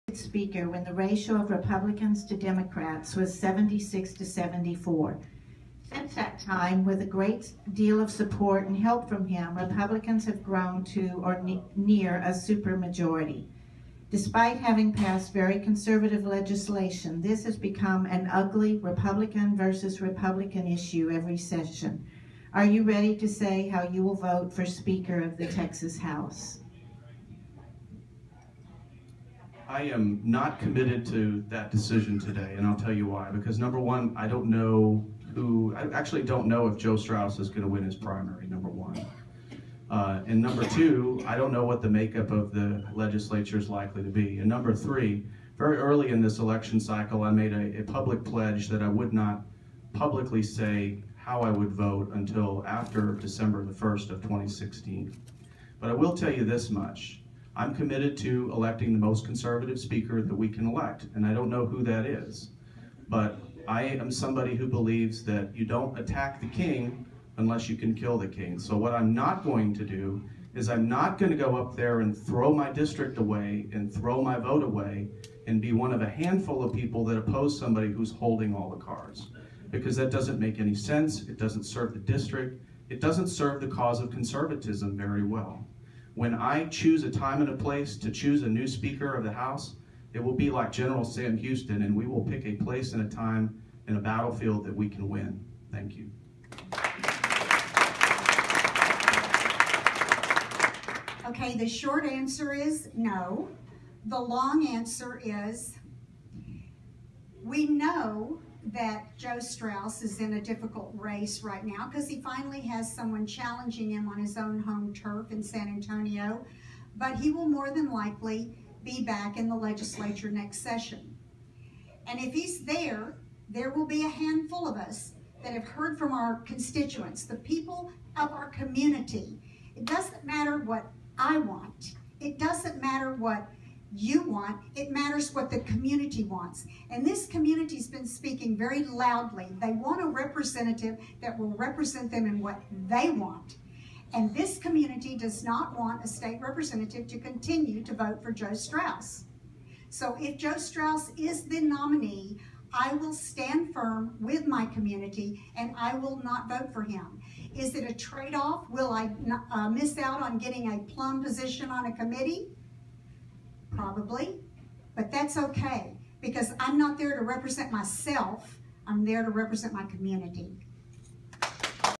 RERvideo - HD130 Candidate Oliverson, "Straus is King"
Hand-picked by his RINO, pro-Straus predecessor, Texas House candidate Dr. Tom Oliverson not only refused to commit to stand against Straus and, the plutocratic status quo, he is recorded calling Texas Speaker Joe Straus, public enemy #1 of liberty in Texas, "THE KING"!

Conversely, liberty candidate Kay Smith is recorded having NO HESITATION stating she will NOT vote for Straus.

Apparently, Smith has more balls than Oliverson.